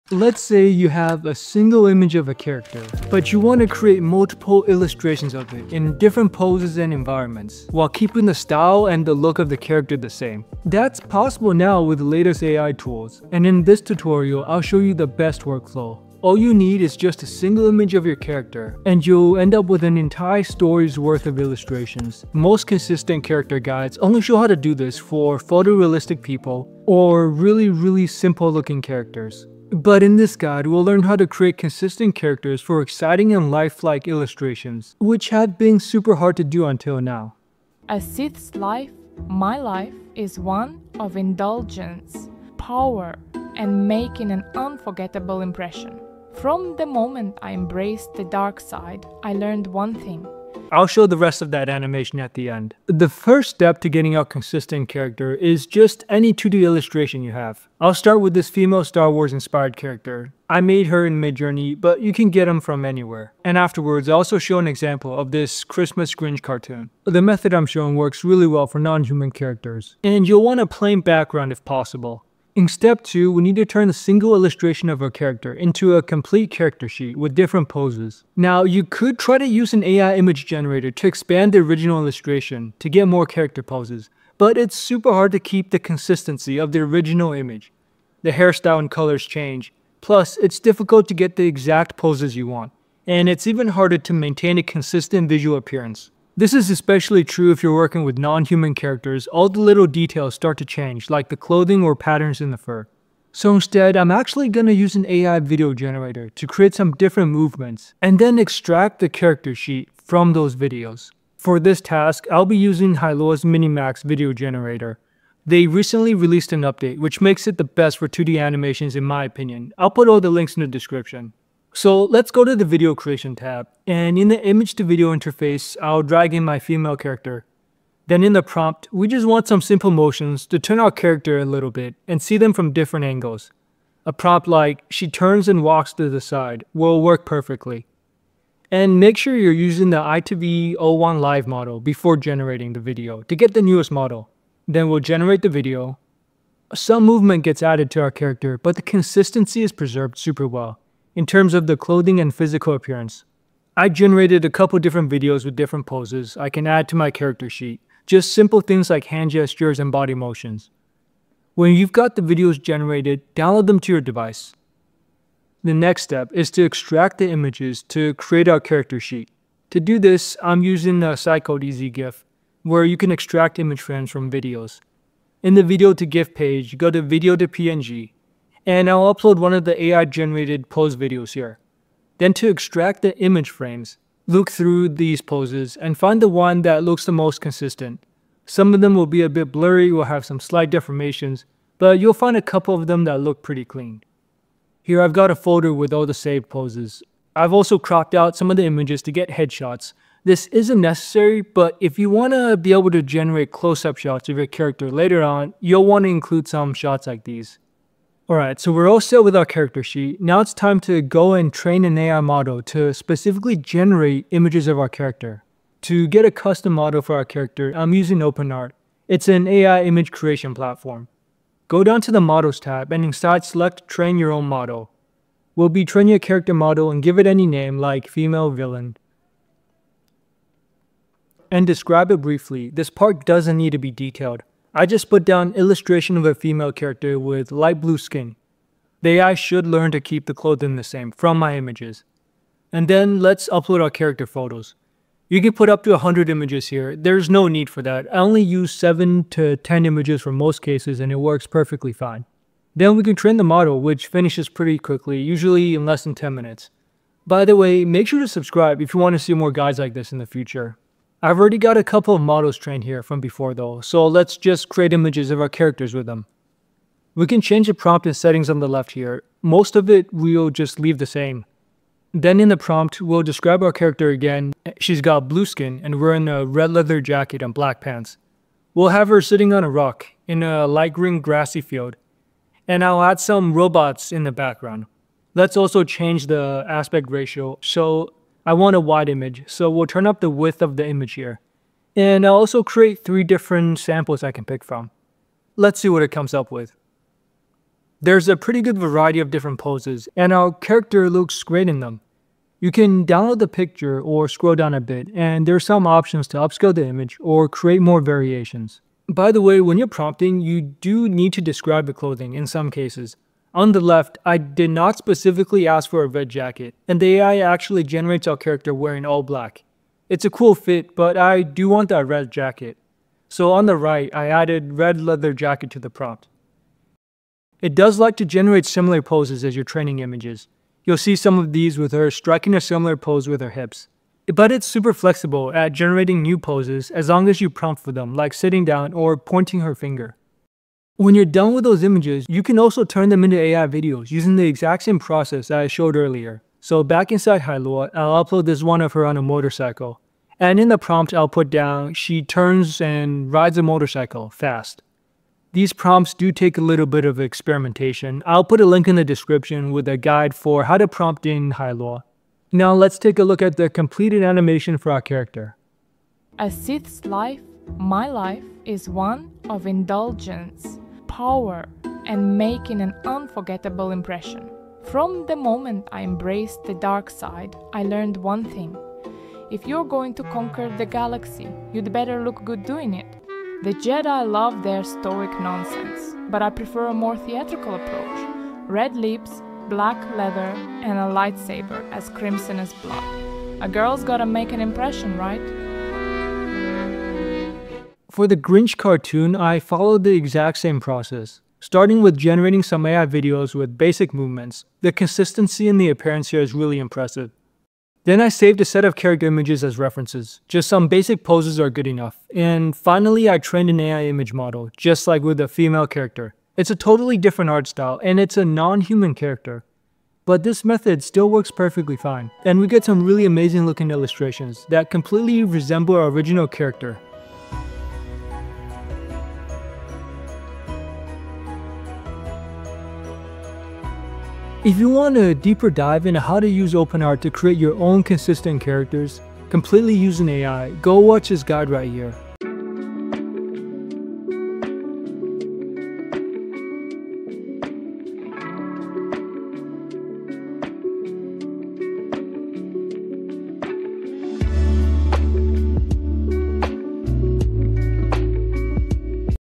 Create Consistent Ai Characters from a Single Image!
Here's how to create consistent characters using only a single image!
We'll start with only a single 2D illustration and train a custom Flux Ai model. We'll explore how to turn it into a consistent character in all sorts of different environments and settings, and create Ai illustrations of our character will many different poses.
We'll even learn how to create an Ai video animation of our character!

Chapters:
00:00 Consistent Ai Characters from Single Image
00:56 Starting Character Reference
01:12 Getting a Character Sheet
03:29 Custom Ai Character Model
05:09 Prompt Tips
05:41 Creating Ai Animations
06:38 2nd Character Example